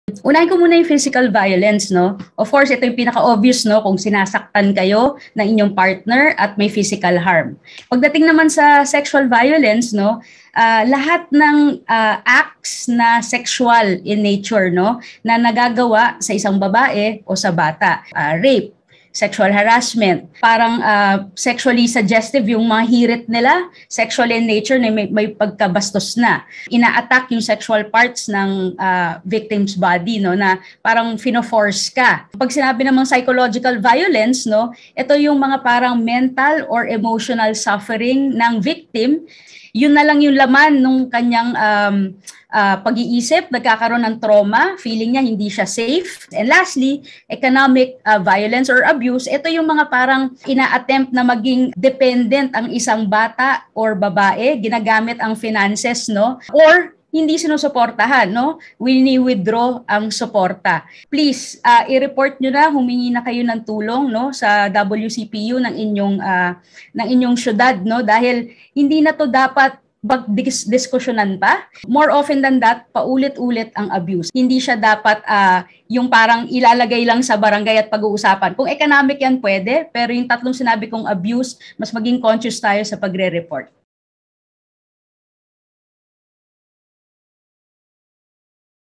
ExperTok: Ano ang iba’t ibang uri ng pang aabuso sa ilalim ng batas
Alam mo bang pwede kang magkaso kung nakakaranas ka ng economic abuse mula sa iyong asawa? Alamin ang iba't iba pang porma ng pang-aabuso mula kay Rea Villa, chief psychologist ng Women and Children Protection Unit ng Pasig City General Hospital.